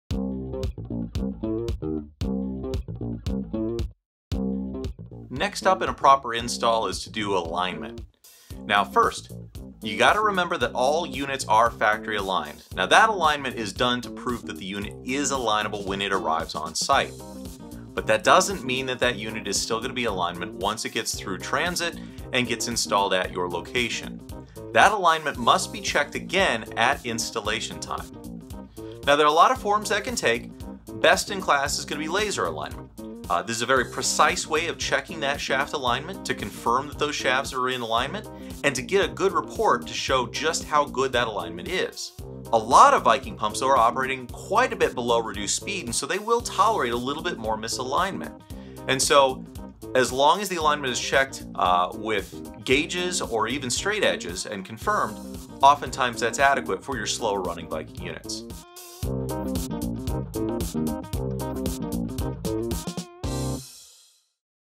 Alignment | Pump Installation and Start Up
(5/7) Viking's own Chad Wunderlich walks through the steps for the proper installation and start up of a Viking pump.

In this episode, we discuss unit alignment - key to a properly functioning pump.

▬ Social Media ▬▬▬▬▬▬▬▬▬▬▬▬▬▬▬